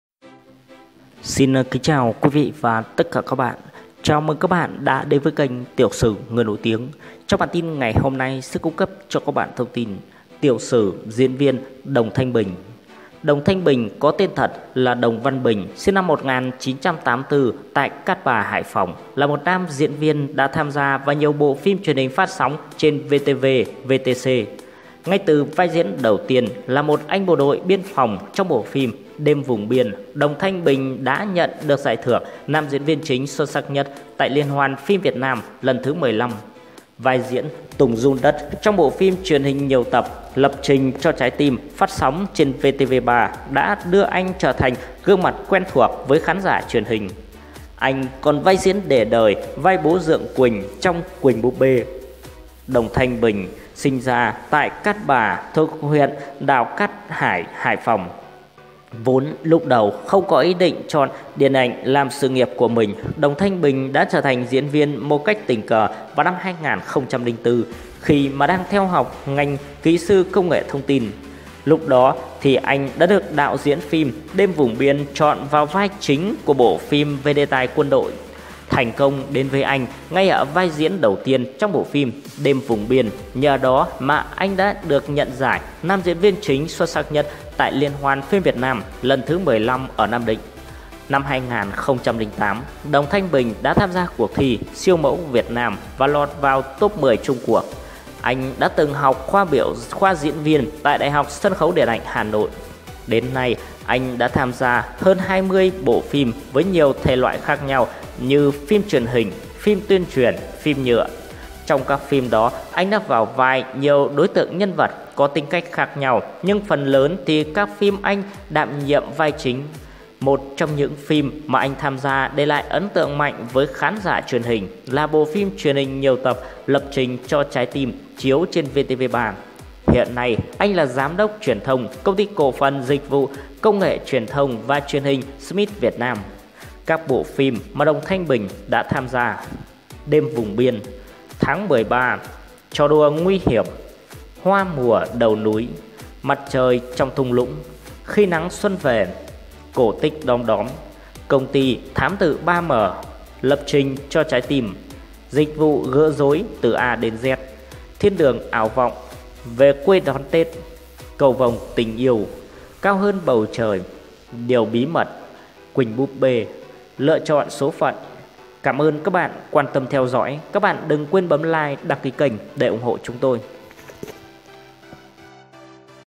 Tiểu Sử Đồng Thanh Bình – Từ Bước Chân Vào Nghề Với Con Số 0 Đến Nổi Bật Trong Quỳnh Búp Bê
Tiểu Sử Diễn Viên Đồng Thanh Bình – Từ Bước Chân Vào Nghề Với Con Số 0 Đến Bố dượng Quỳnh trong Quỳnh búp bê
Đồng Thanh Bình tên thật là Đồng Văn Bình (sinh năm 1984 tại Cát Bà, Hải Phòng), là một nam diễn viên đã tham gia trong nhiều bộ phim truyền hình phát sóng trên VTV, VTC. Ngay từ vai diễn đầu tiên là một anh bộ đội Biên phòng trong phim "Đêm vùng biên", Đồng Thanh Bình đã được nhận giải thưởng Nam diễn viên chính xuất sắc nhất tại Liên hoan phim Việt Nam lần thứ XV. Vai diễn Tùng giun đất trong bộ phim truyền hình nhiều tập "Lập trình cho trái tim" phát sóng trên VTV3 đã đưa anh trở thành gương mặt quen thuộc với nhiều khán giả truyền hình. Anh còn vai diễn để đời là vai Bố dượng Quỳnh trong Quỳnh búp bê.